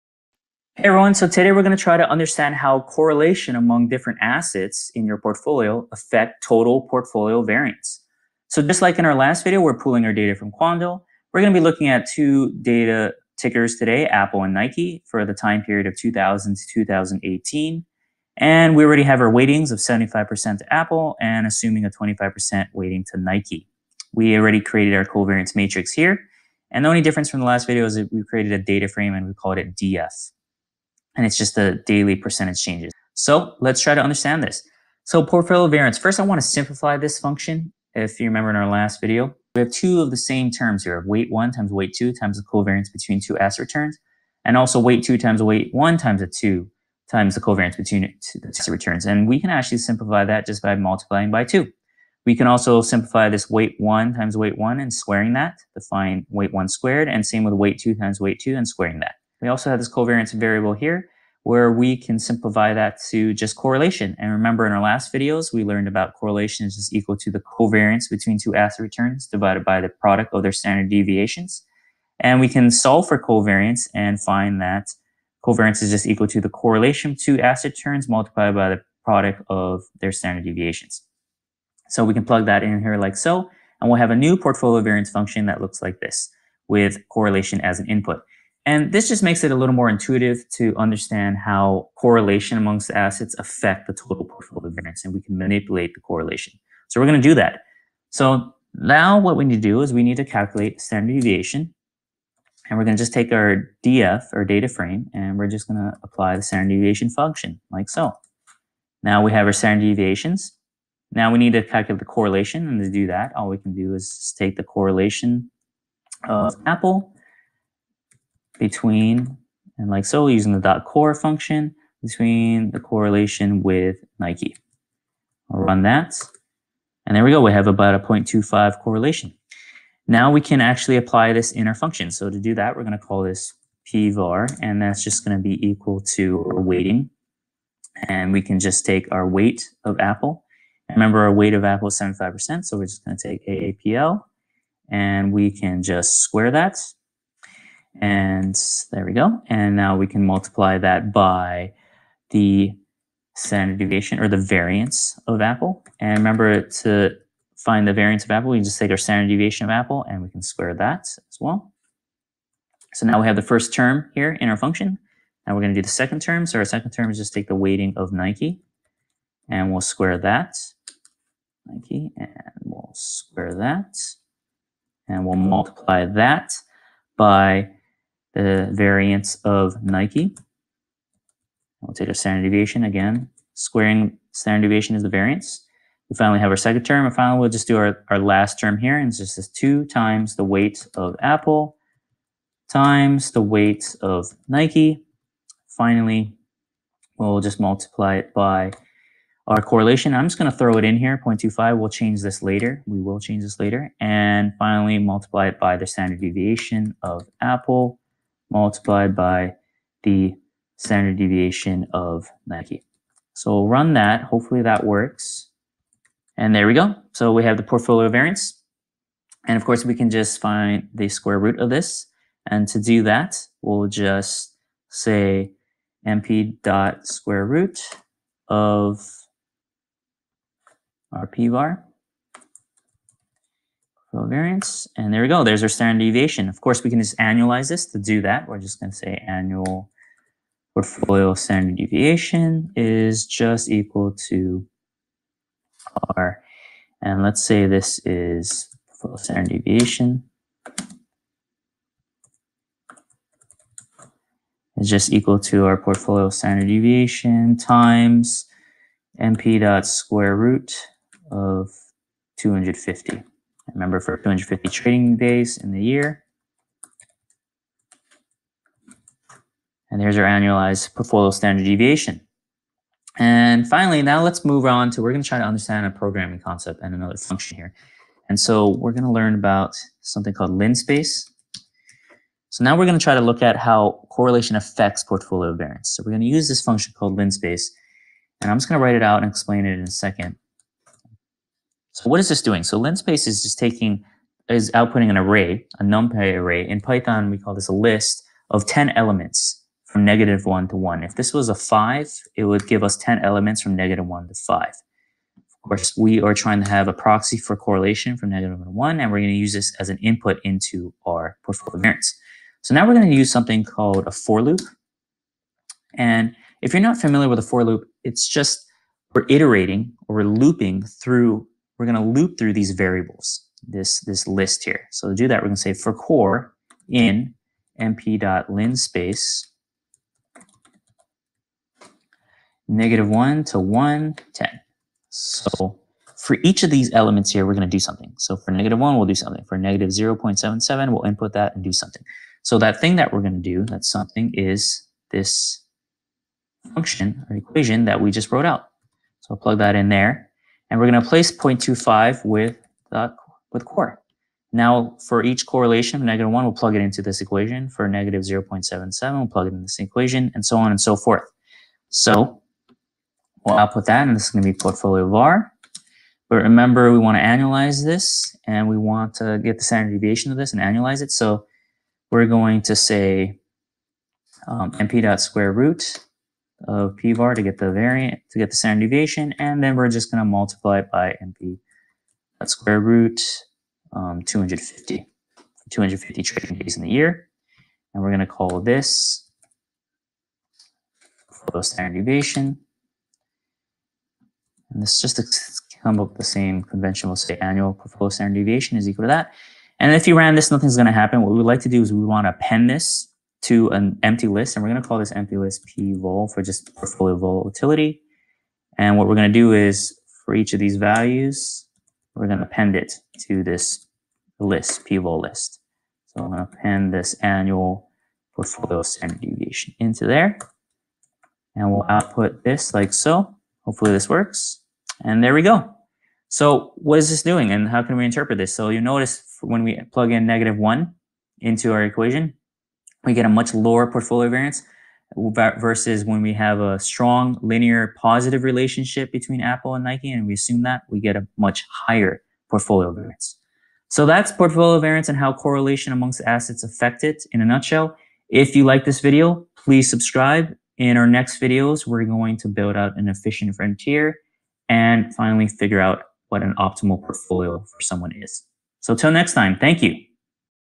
How to calculate portfolio variance & volatility in Python? Part II
In this video we learn the fundamentals of calculating portfolio variance. This will help us in our quest to constructing an efficient frontier, building a portfolio optimizer and eventually finding an investors optimal portfolio.